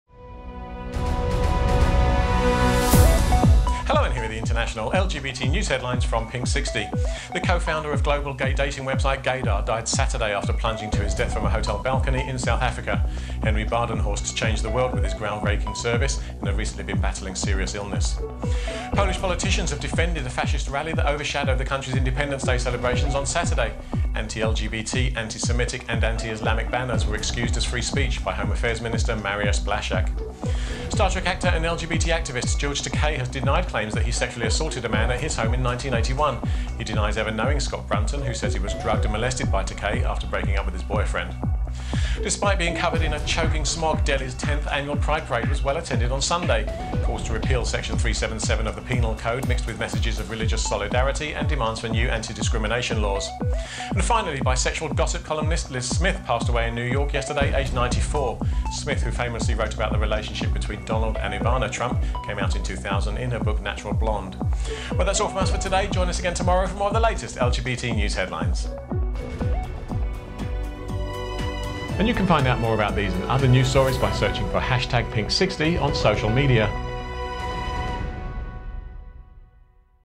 FASCISTS RISE OVER POLAND
Today's News from Pinksixty.com

The co-founder of global gay dating website, Gaydar died Saturday after plunging to his death from a hotel balcony in South Africa. Henry Badenhorst changed the world with his groundbreaking service and had recently been battling serious illness.

Polish politicians have defended a fascist rally that overshadowed the country’s Independence Day celebrations on Saturday. Anti-LGBT, antisemitic and anti-Islamic banners were excused as ‘free-speech’ by Home Affairs Minister Mariusz Blaszczak.

Star Trek actor and LGBT activist, George Takei has denied claims that he sexually assaulted a man at his home in 1981. He denies ever knowing Scott Brunton, who says he was drugged and molested by Takei after breaking up with his boyfriend.

Despite being covered in a choking smog, Delhi's 10th annual Pride Parade was well attended on Sunday. Calls to repeal Section 377 of the penal code mixed with messages of religious solidarity and demands for new anti-discrimination laws.

And finally, bisexual gossip columnist Liz Smith passed away in New York yesterday aged 94. Smith who famously wrote about the relationship between Donald & Ivana Trump, came out in 2000 in her book, 'Natural Blonde'.

Join us on the SOCIAL

Help Make us Grow!

Ms Yemi King - (Web mistress, social media & Branding)